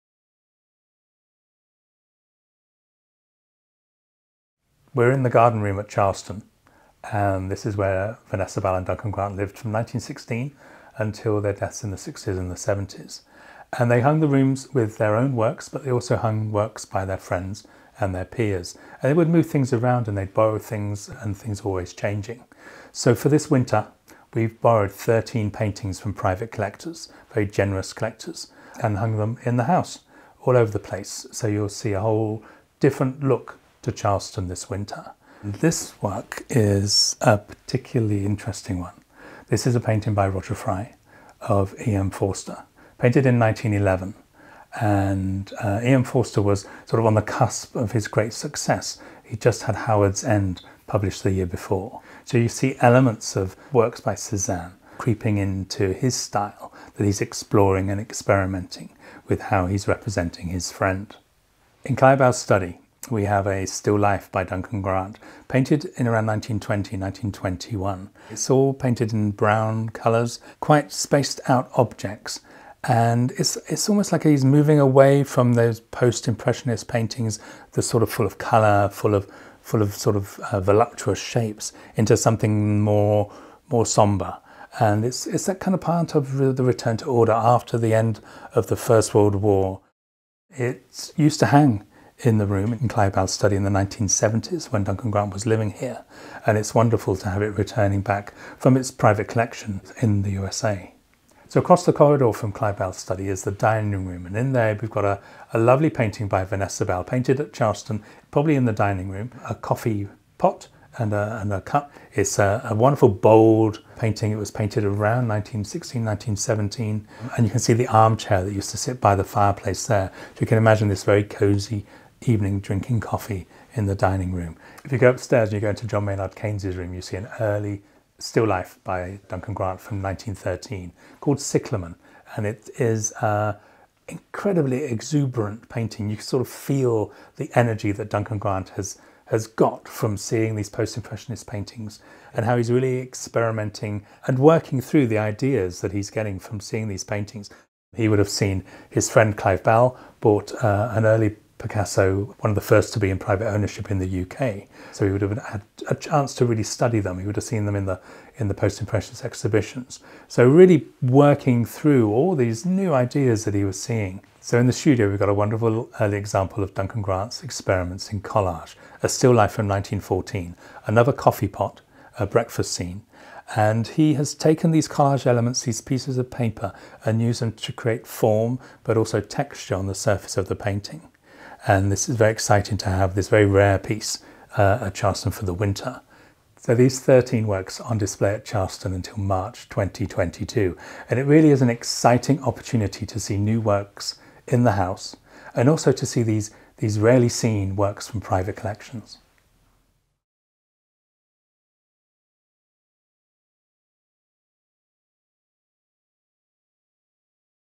House Rehang: 13 exciting new loans on display
This autumn and winter we have 13 new artworks on display in the house, generously loaned to us from private collections. It’s a rare opportunity to see new works in the house and among the highlights are a full length nude by Duncan Grant in the library, a portrait of Vanessa Bell by the artist in his bedroom, a selection of still lifes by both Bell and Grant, and a striking portrait of the writer E. M. Forster by Roger Fry.

This rehang also includes rarely exhibited works from our own collection, and will be on display until 13 March 2022.